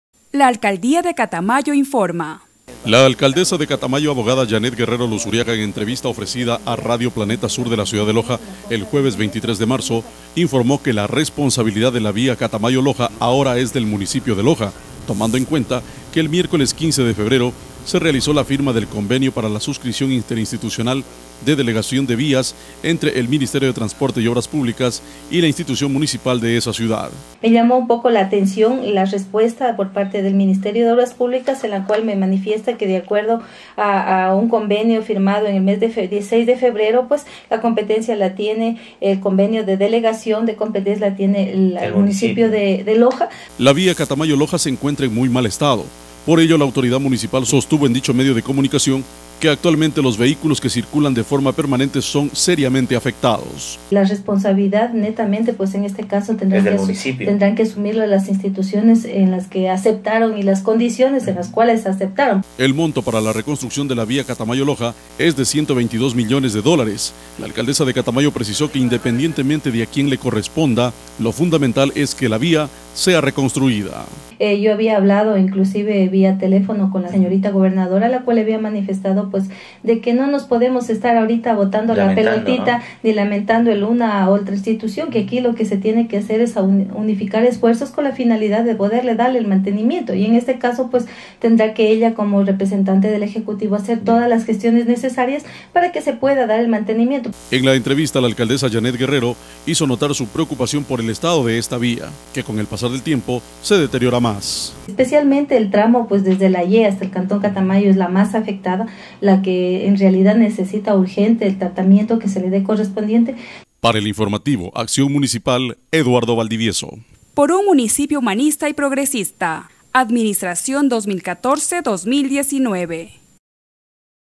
Alcaldesa de Catamayo habla sobre la competencia de la vía Catamayo Loja
La Alcaldesa de Catamayo Abogada Janet Guerrero Luzuriaga, en entrevista ofrecida a radio Planeta Sur de la ciudad de Loja, el jueves 23 de marzo informo que la responsabilidad de la vía Catamayo Loja, ahora es del Municipio de Loja, tomando en cuenta que el miércoles 15 de febrero, se realizó la firma del convenio para la suscripción interinstitucional de la delegación de vías entre Ministerio de Transporte y Obras Públicas (MTOP) y la institución municipal de esa ciudad.
La vía Catamayo Loja se encuentra en muy mal estado, por ello la autoridad municipal sostuvo en dicho medio de comunicación, que actualmente los vehículos que circulan de forma permanente, son seriamente afectados.
El monto para la reconstrucción de la vía Catamayo Loja es de ciento veinte y dos millones de dólares, la Alcaldesa de Catamayo, precisó que independientemente de a quien le corresponda, lo fundamental es que la vía sea reconstruida.
En la entrevista la Alcaldesa Janet Guerrero hizo notar su preocupación por el estado de la vía, que con el pasar del tiempo se deteriora más.